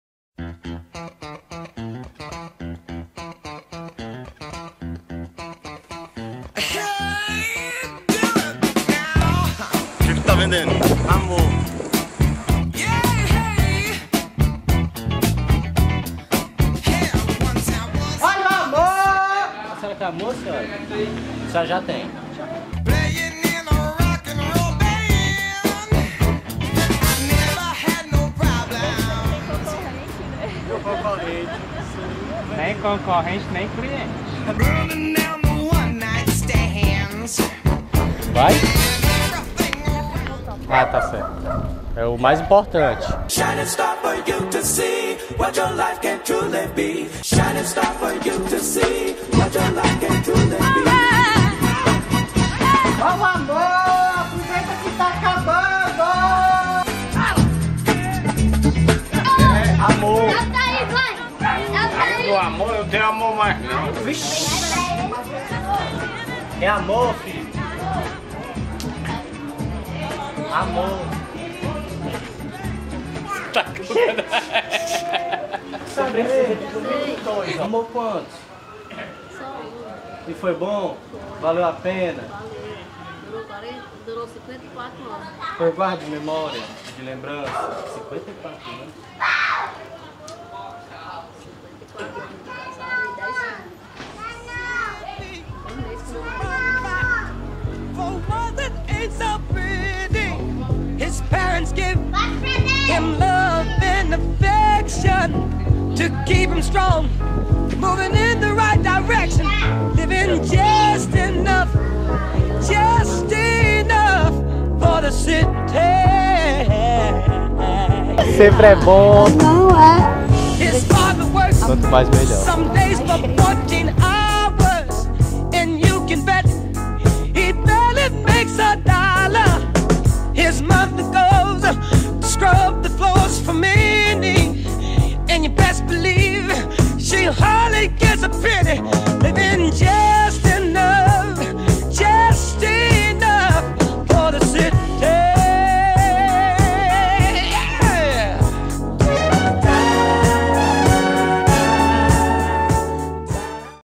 O VENDEDOR DE AMOR (TEASER) - As 10 Graças
Performance criada e executada pelo artista de rua João Victor.
Produção: Alysson Lemos.
Imagens: Eduardo Cunha.
Edição: Igor Cândido.